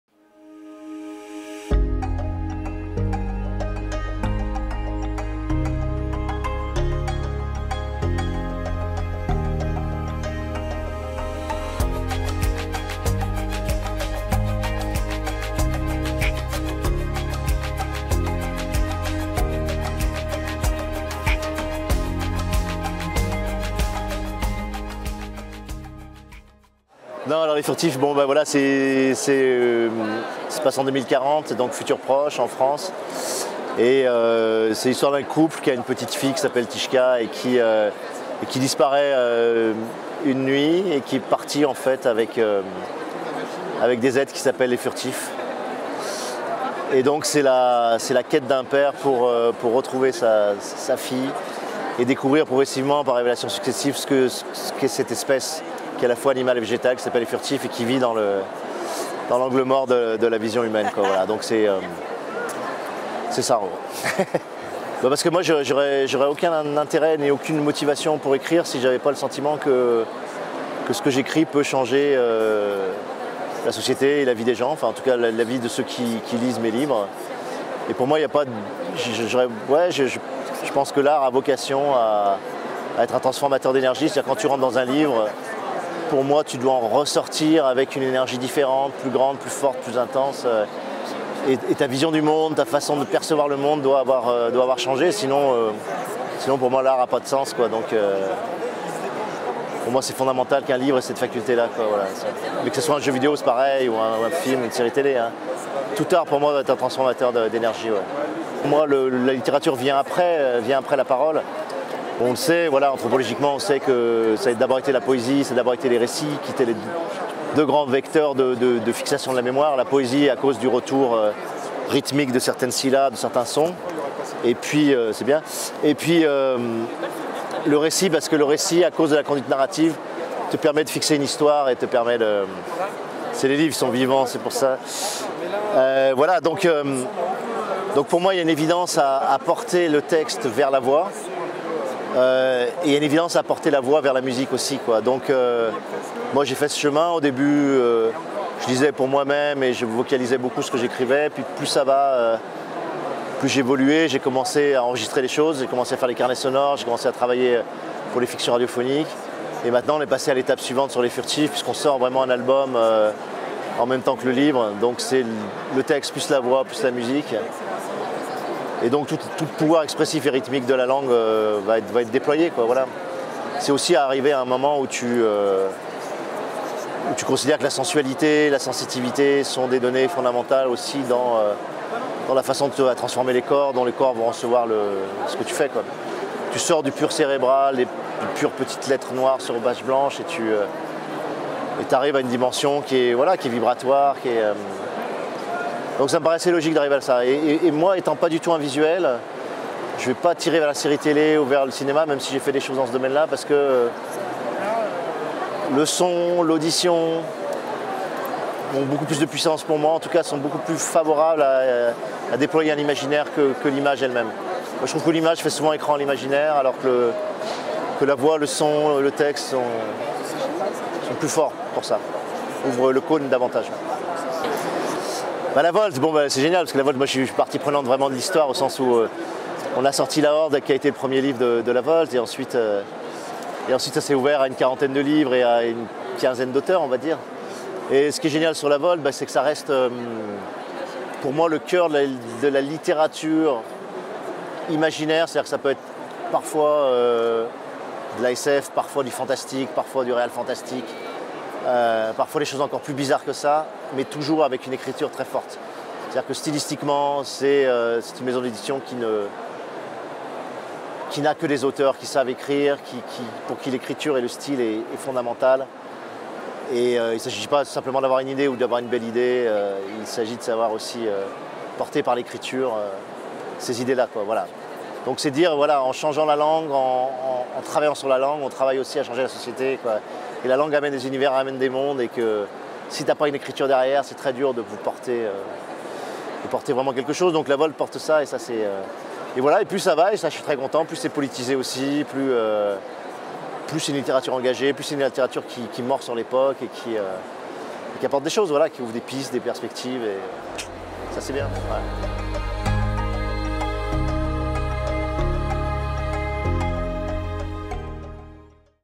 Alain Damasio - Les Furtifs
A l'occasion de Livre Paris 2019, rencontre avec Alain Damasio autour de son ouvrage "Les Furtifs" aux éditions La Volte.